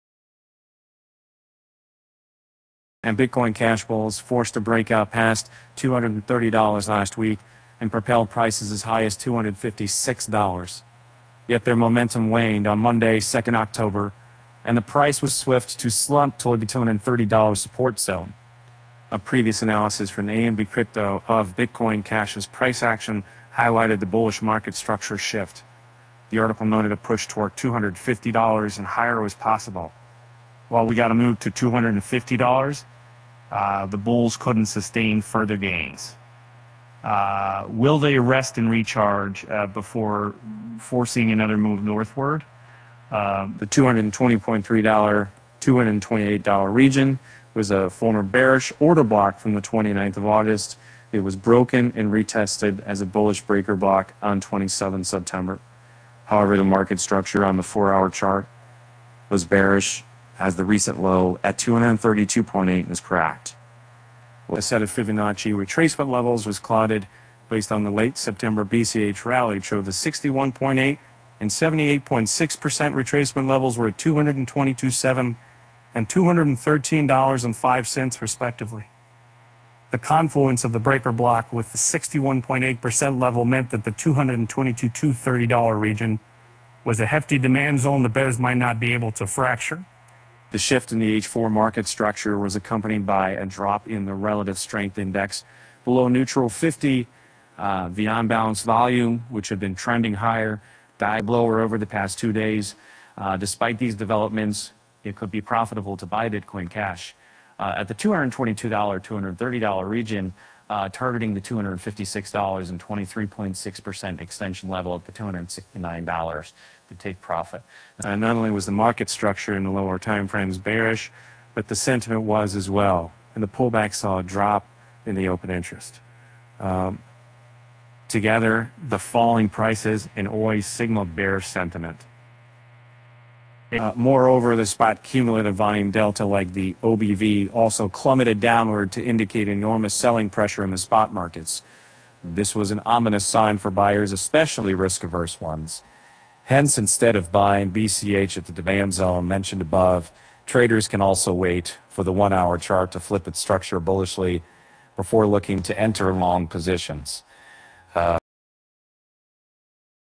BCH faced rejection at $250; here’s why the bulls can try again and succeed
Bitcoin Cash bulls forced a breakout past $230 last week and propelled prices as high as $256. Yet, their momentum waned on Monday, 2 October and the price was swift to slump toward the $230 support zone.